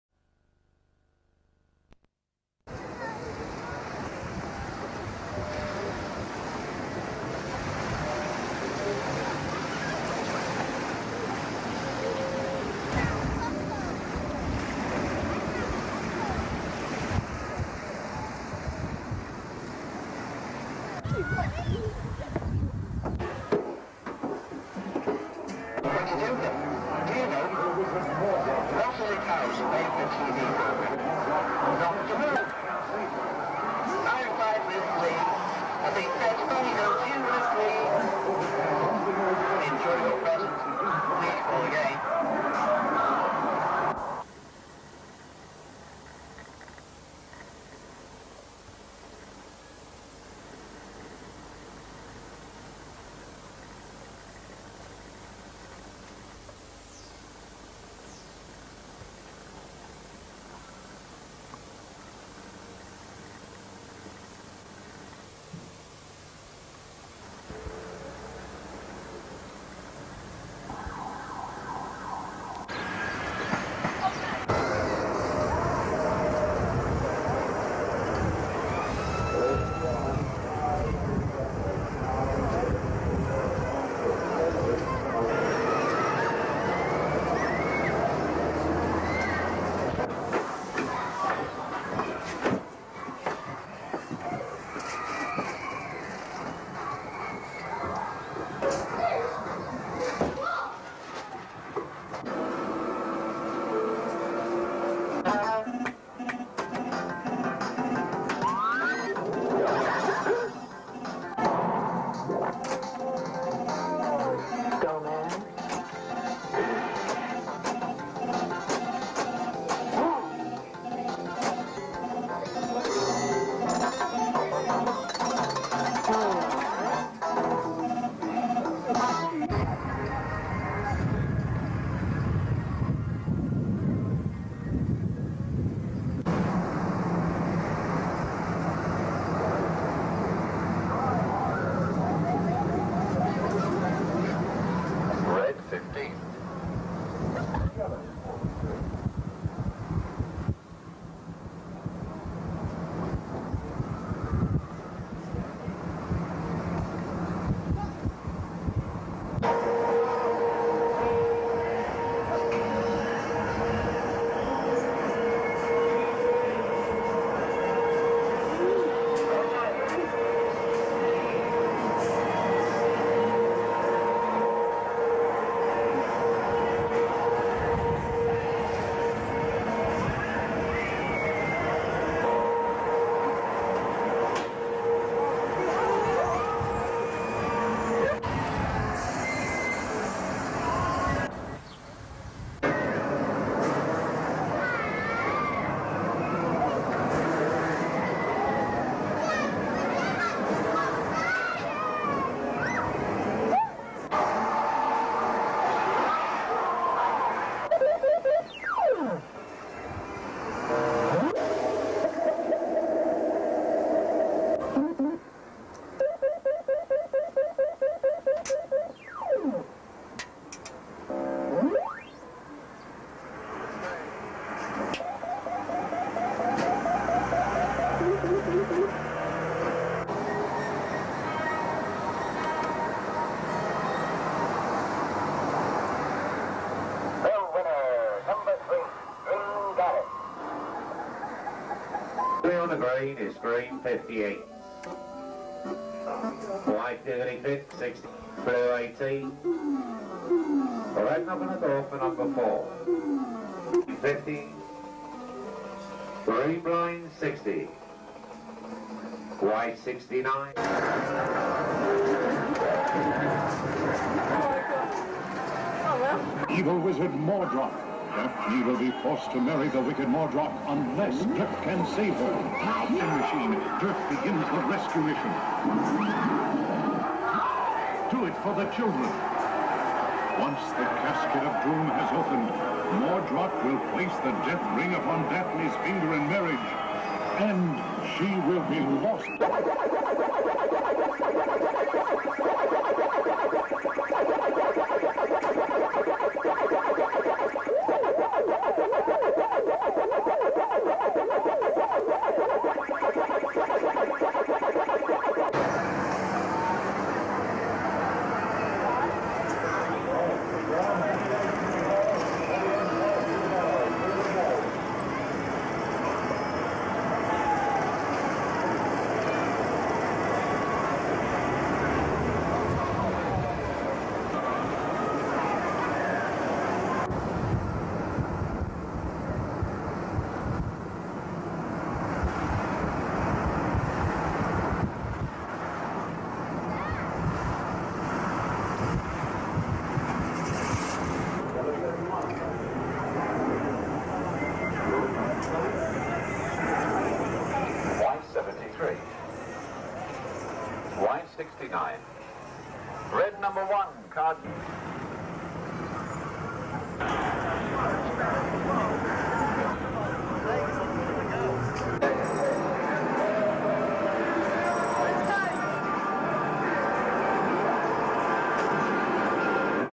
Southend on sea, Essex, UK - picture postcard
UK Short Film. Southend on Sea. Seaside.

Dear.

We went to Southend and made castles out of sand and seashells. I bought you a stick of your favourite stripy rock.  We took a ride on the big wheel, I'm not afraid of heights, but it's scarier than you think as it was suspended for a while, and I found it hard to open my eyes.

I played bingo but didn't win anything, although I did win a prize on one of those crane machines on the first try.